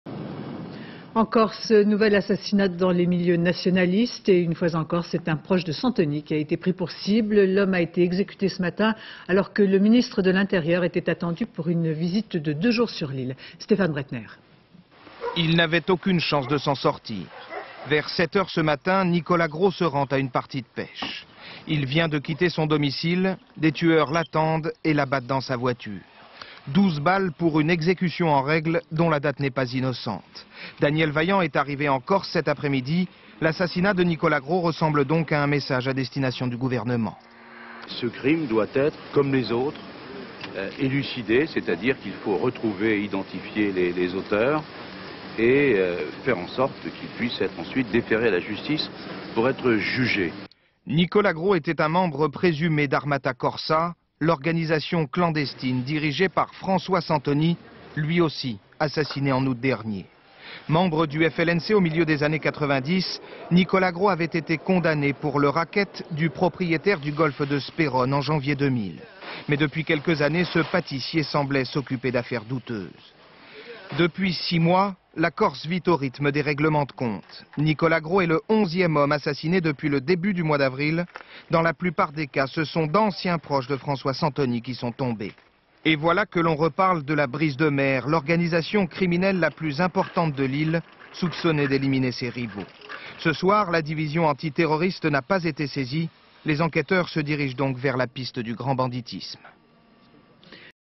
[Assassinat Nicolas Gros Bonifacio]
26 octobre 2001
En Corse, nouvel assassinat dans les milieux nationalistes. Une fois encore c'est un proche de François Santoni qui a été pris pour cible. L'homme a été exécuté ce matin, alors que le Ministre de l'Intérieur était attendu pour une visite de deux jours sur l'ile.Stephane Breitner. ... politique; archive television; archive tv; ina; inna; Institut National de l'Audiovisuel; french tv Images d'archive INA